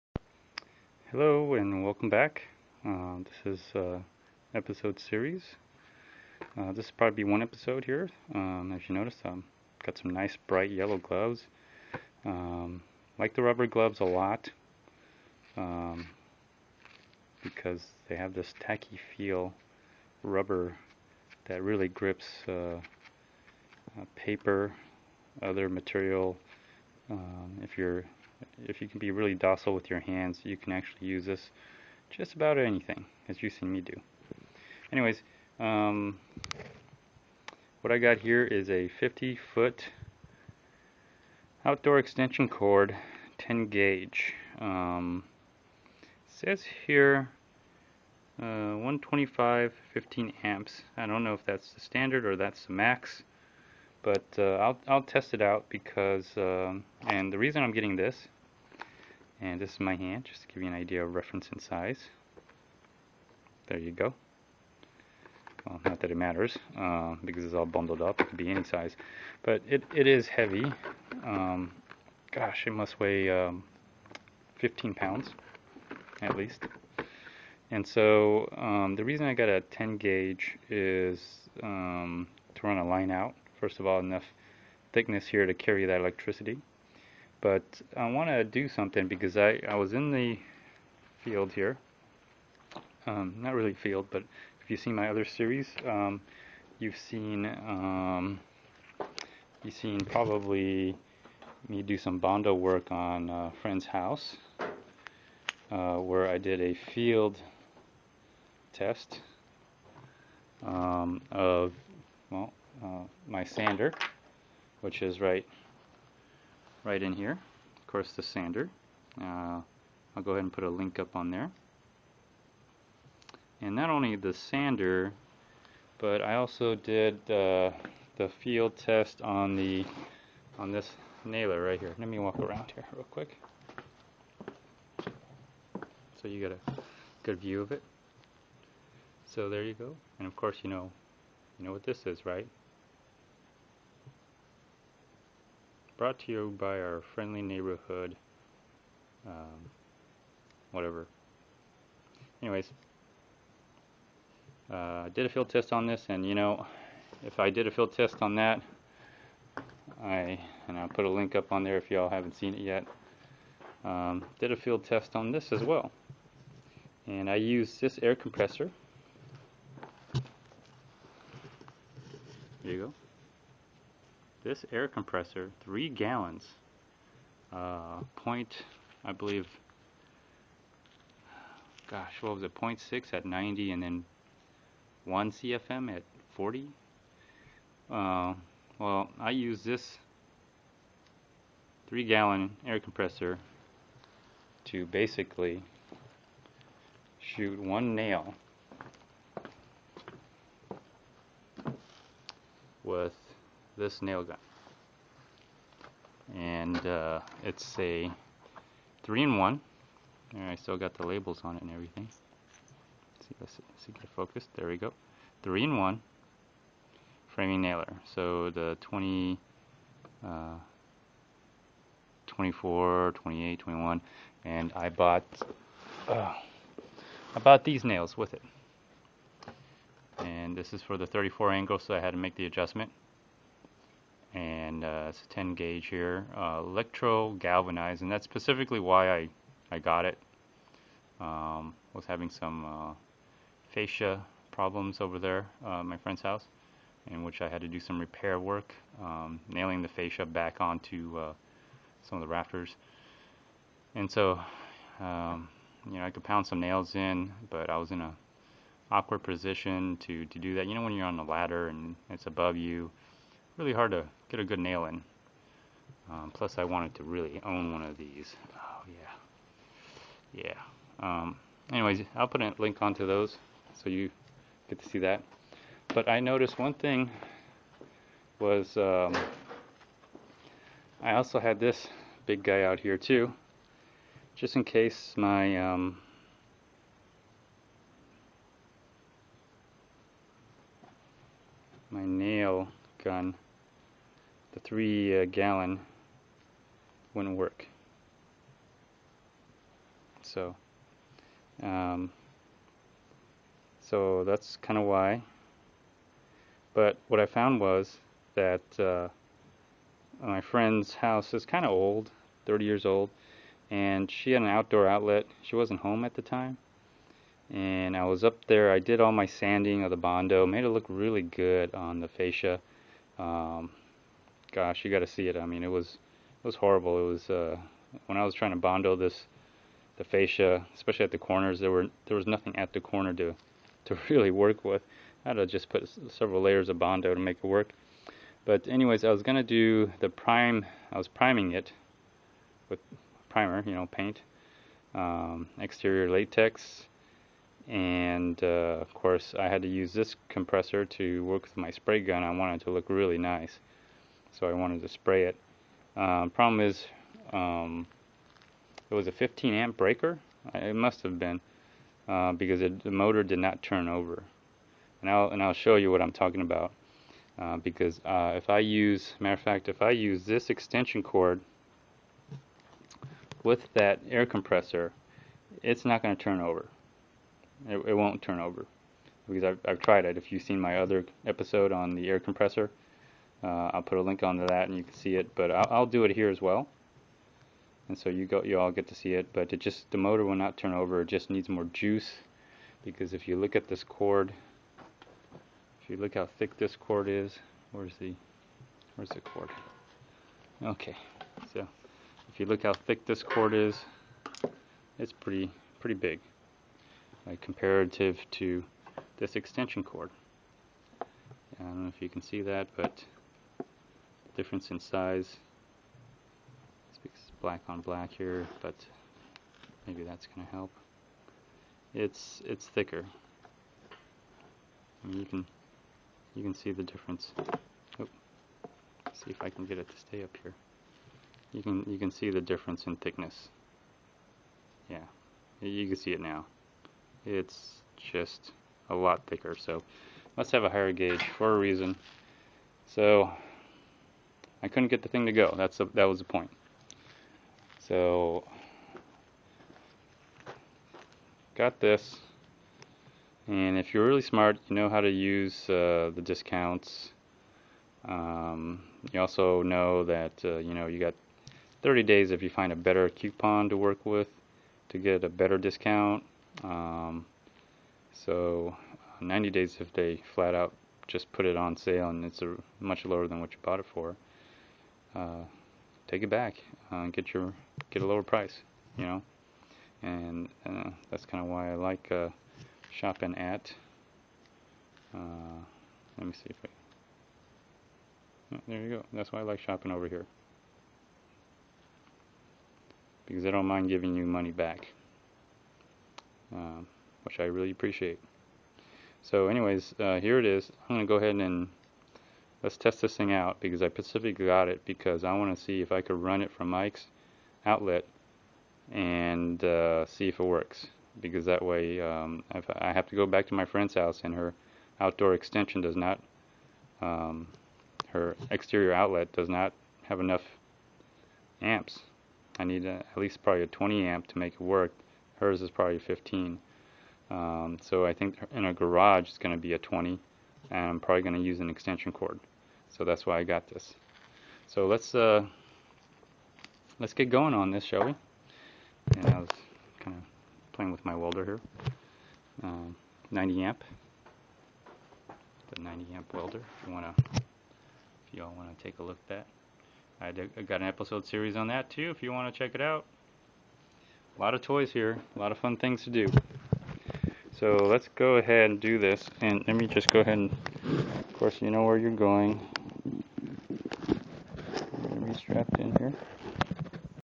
10 Gauge 50 Feet Extension Cord Part 01
In this episode series I find many good uses for my new extension cord.  Like powering my air compressor!!!  As always uncut, unedited, raw footage in first person view.